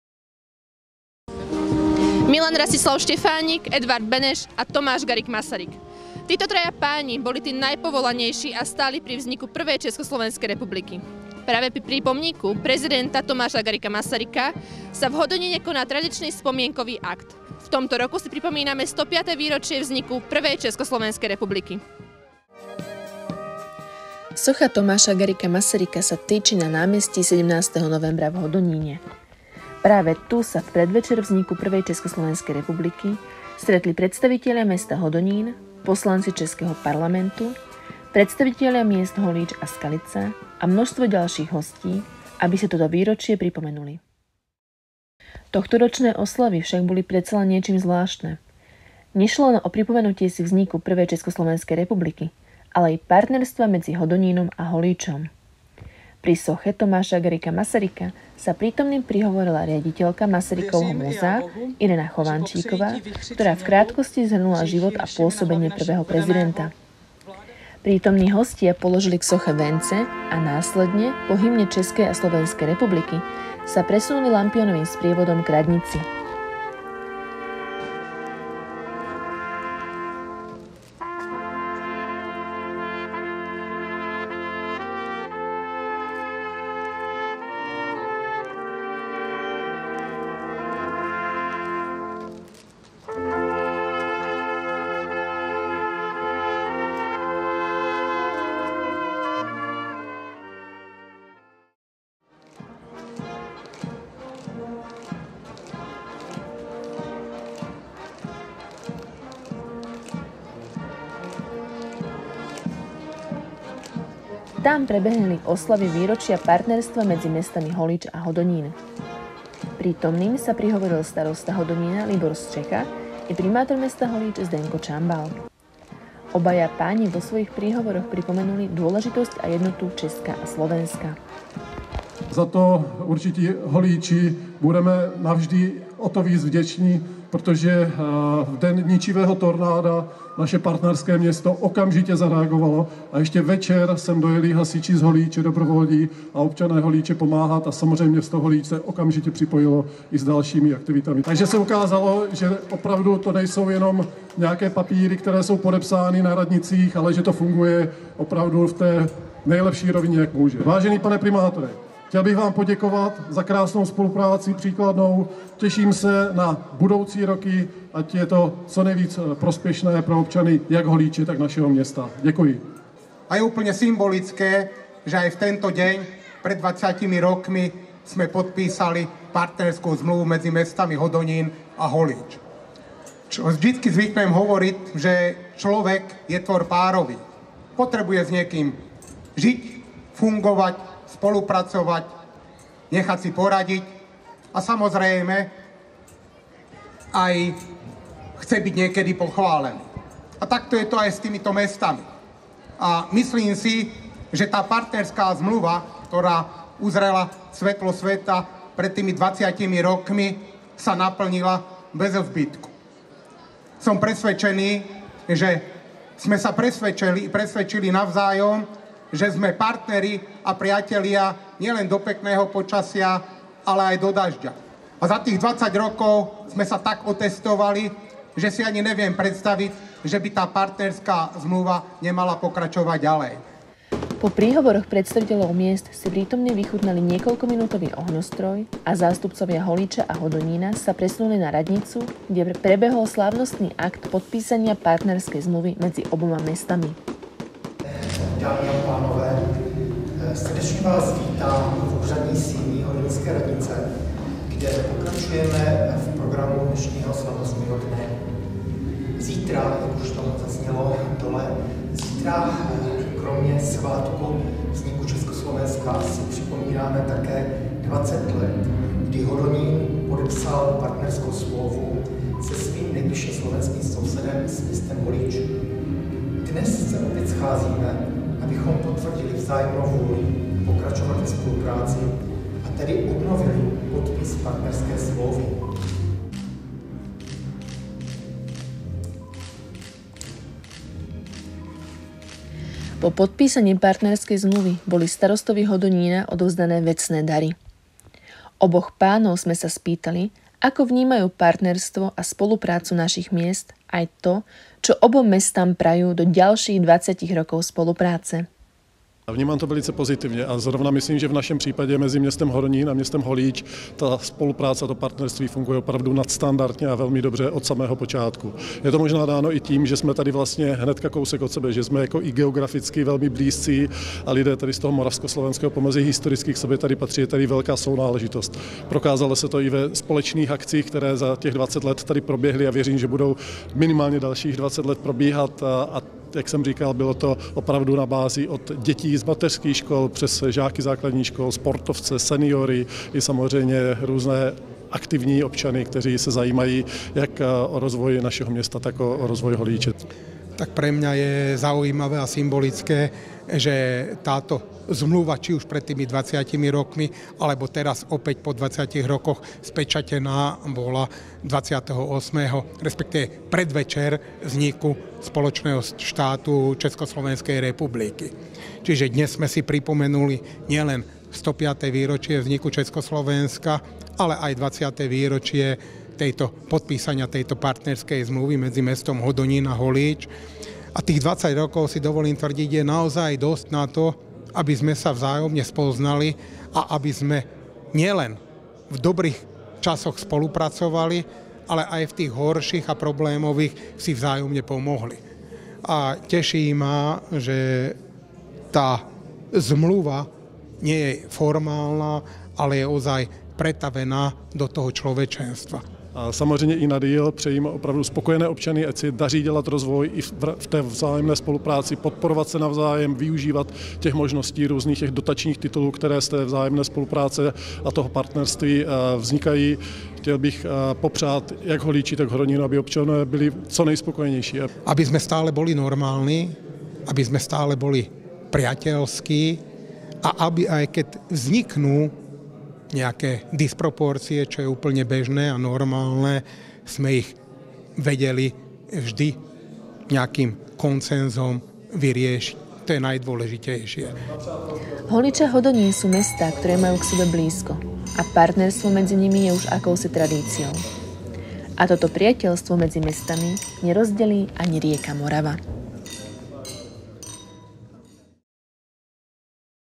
105. výročie vzniku ČSR
28. októbra sme si pripomenuli 105. výročie vzniku prvej Československej republiky. Pri tejto príležitosti sa v Hodoníne uskutočnilo pri soche prvého prezidenta Tomáša G. Masaryka spomienkové podujatie. Súčasťou programu bolo aj pripomenutie si 20. výročia partnerstva medzi Holíčom a Hodonínom. Na znak vzájomnej spolupráce najvyšší predstavitelia oboch miest podpísali dohodu o partnerstve.